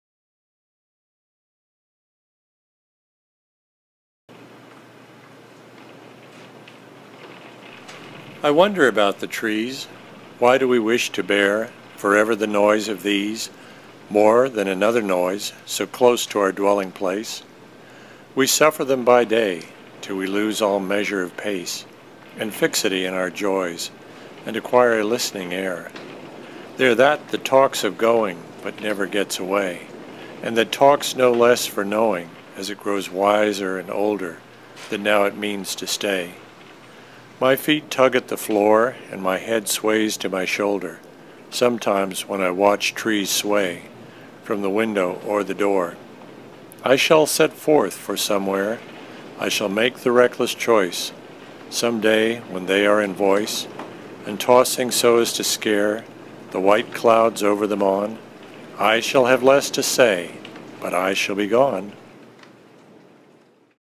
Robert Frost: The Sound of Trees
Sometimes a scene in nature will evoke a phrase or a poem that I have learned. One of my favorite poets is Robert Frost, and long ago I memorized his "The Sound of Trees." One day the wind in the trees outside my house in Cheney, Washington, reminded me of this poem, and so I filmed the scene and read in the words.